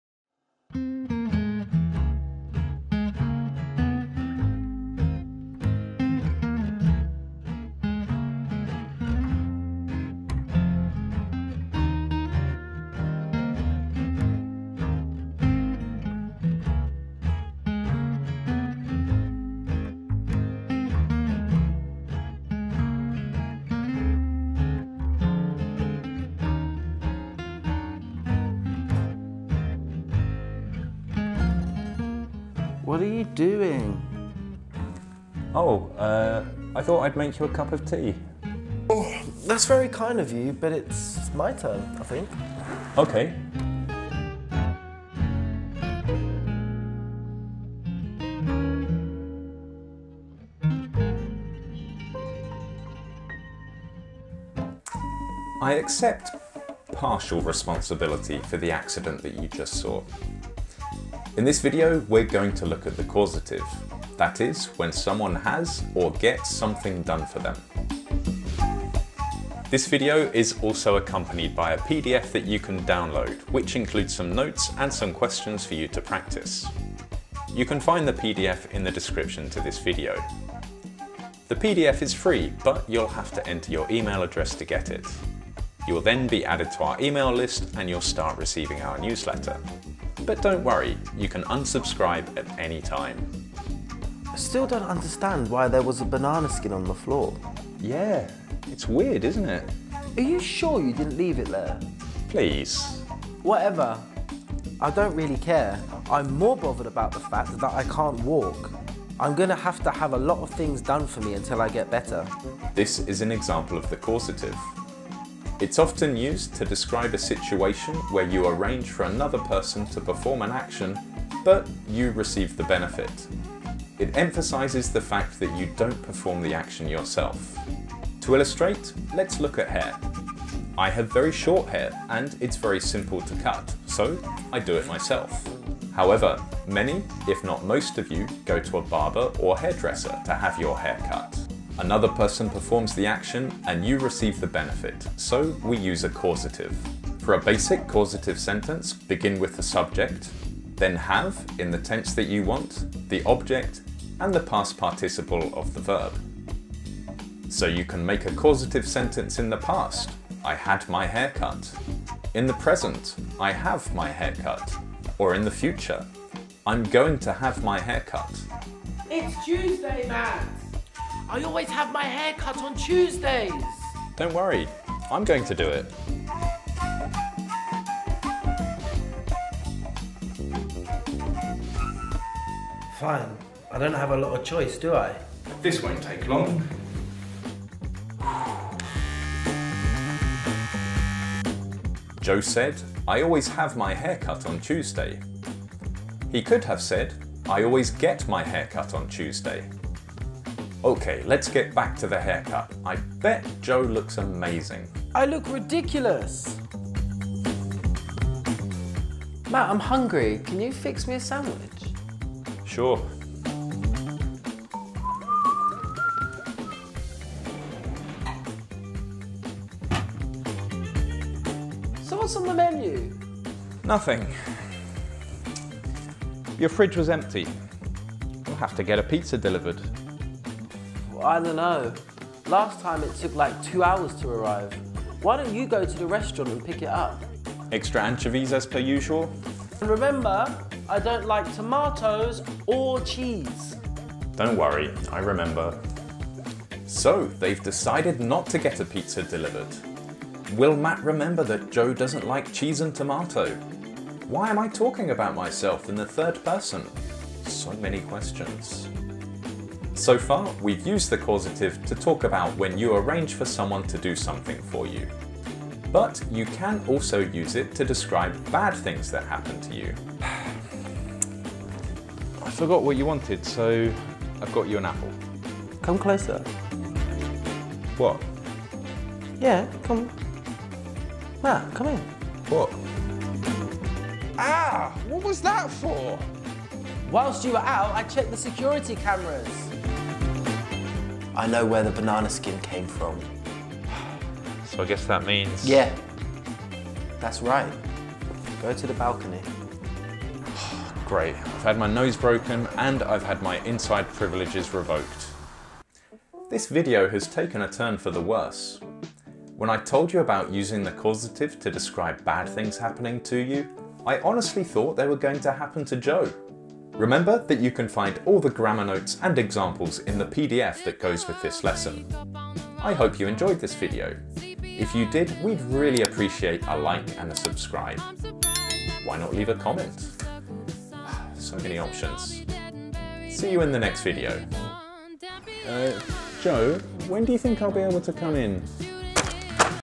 Have/get something done + FREE PDF | B2 English grammar lesson
We use the causative when we arrange for another person to do something for us. We have (or get) something done. Watch the video and download the notes!